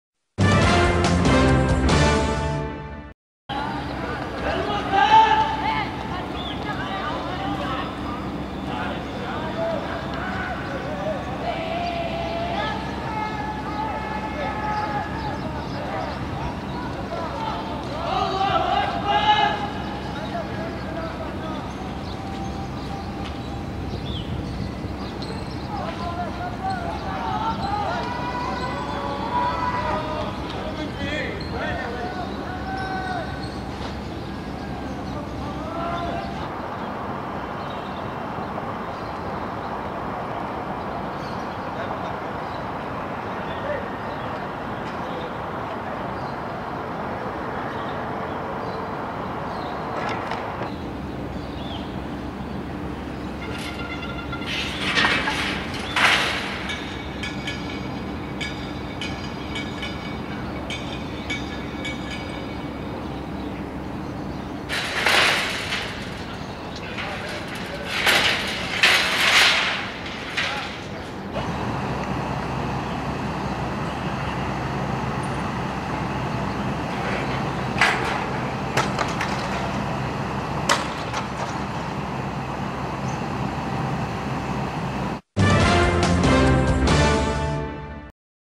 انتشار واسع لمرتزقة النظام صباحا - النويدرات 14/3/2013
انتشار موسع لمرتزقة النظام الخليفي تزامنا مع انطلاق اولى الجوالات الميدانية في الإضراب العام "اضراب الكرامة 2. النويدرات صباح اليوم الخميس (14 مارس/أذار 2013) الساعة 8:02 صباحاً.